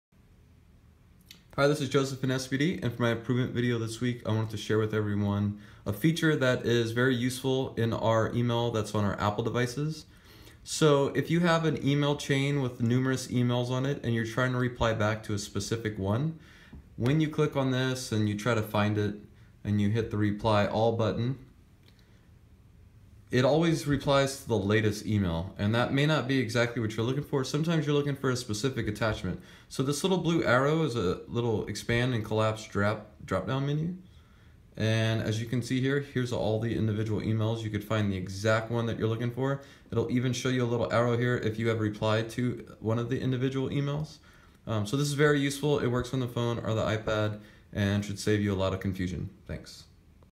Viewing Emails on iPhone - Jmayard
A quick tip to overcome the collapsing of emails in a thread.